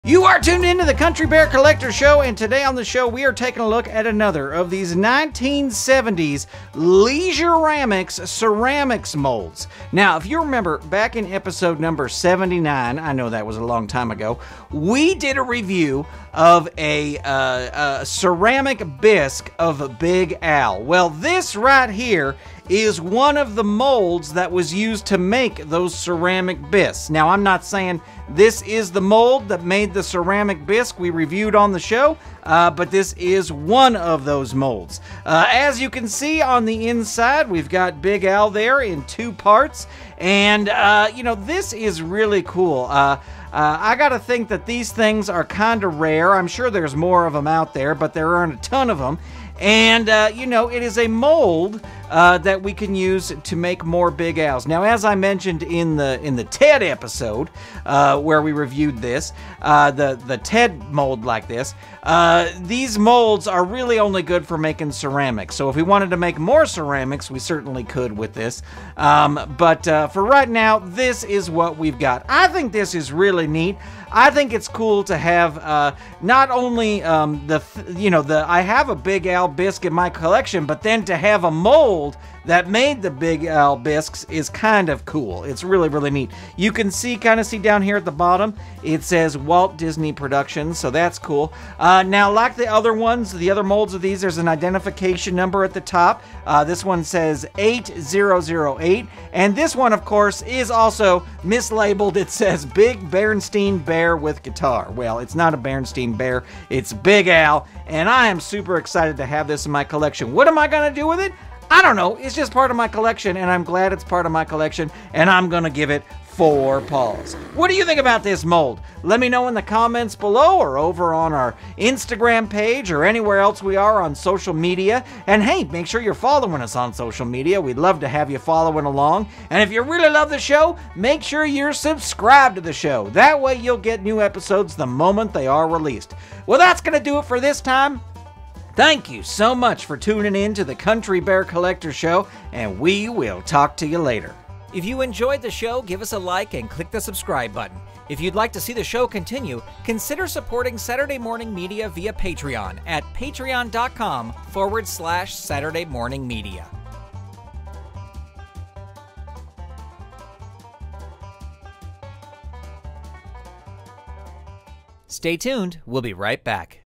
1970s Walt Disney Big Al Leisuramics Ceramics Mold - CBCS 315
- Check out this vintage, 1970s Big Al Ceramic Bisque mold by Leisuramics! Our host gives it a review and then delivers his patented 'Paw Ratings.' Be sure to follow the Country Bear Collector Show Instagram Page over and let us know your thoughts on this item!

PRODUCT INFORMATION
1970s Walt Disney Big Al Leisuramics Ceramics Mold

Episode Edited by Stephen Staver